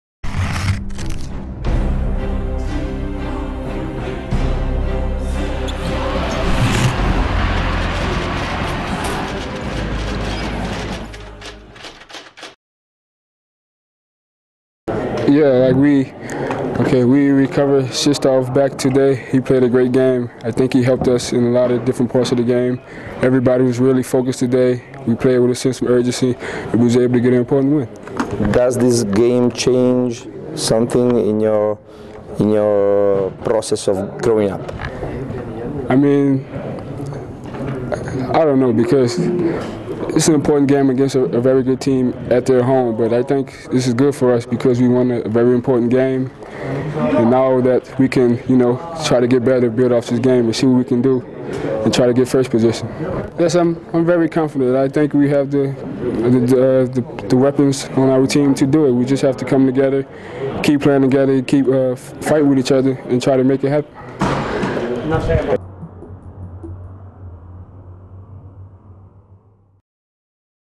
Real - Montepaschi: Bo Mc Calebb
SPECIALE TOP 16 - Il commento del play biancoverde